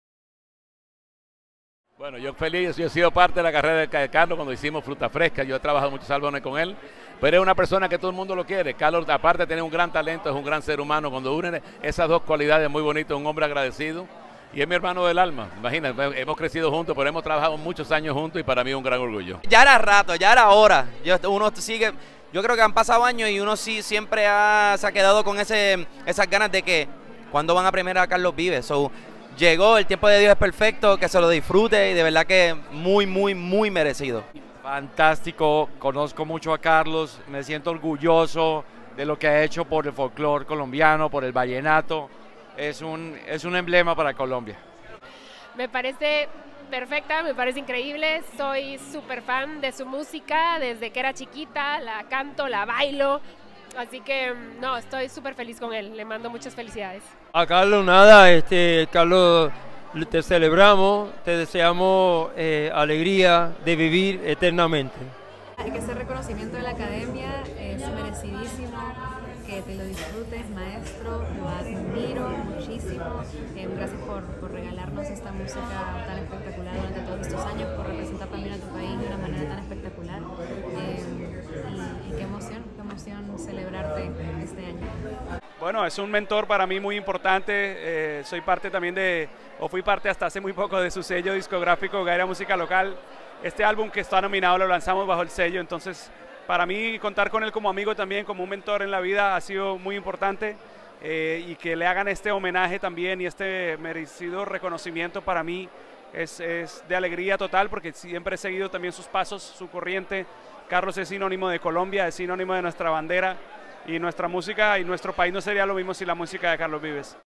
El mensaje de los artistas a la Persona del Año 2024 Carlos Vives
Celebridades como Emilio Estefan, Draco Rosa, Gaby Moreno, Nicole Zignago, Gusi y el multiganador del Latin Grammy Julio Reyes Copello, reaccionan tras la designación de Carlos Vives como Persona del Año 2024 de La Academia Latina de la Grabación.